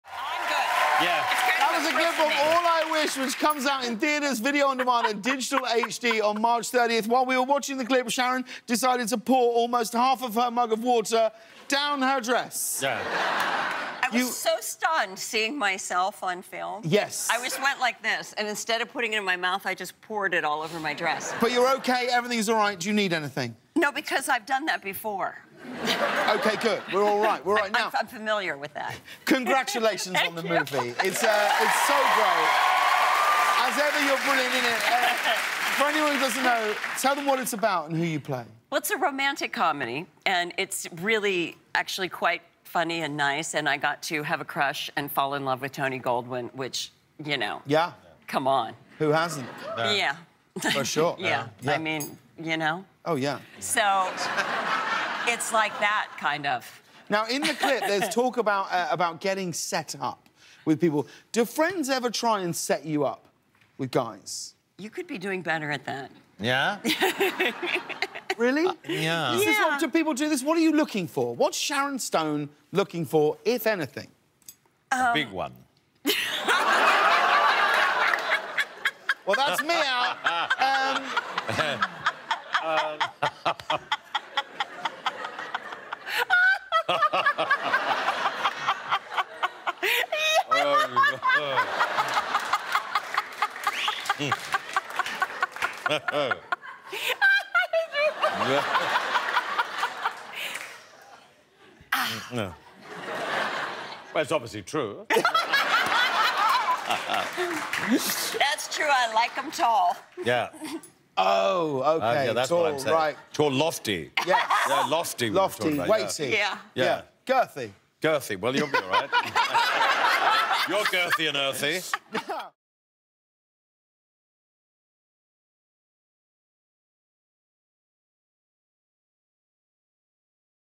What Kind of Man Is Sharon Stone Seeking?
Elton John is quick with an answer to James asking Sharon Stone about friends setting her up on dates and what she's looking for in a man.